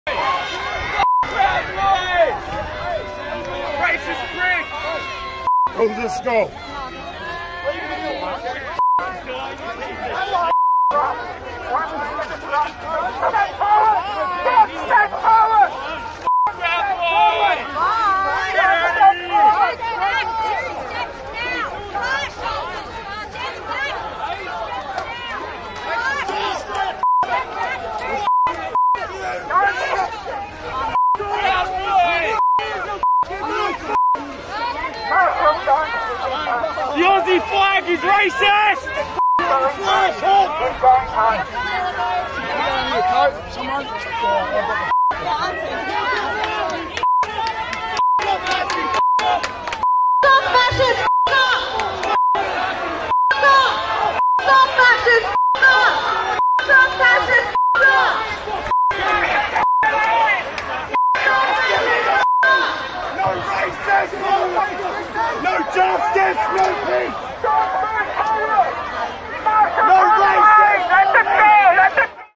Protestors clash with police at Invasion Day rally in Melbourne
Aboriginal activists demand '$1million for every black person' and to be 'liberated from this country' as thousands swarm 'Invasion Day' rallies around Australia
Violence erupts at Invasion Day rally in Melbourne as protestors clash with group appearing to support Proud Boys on Australia Day as police try to maintain peace.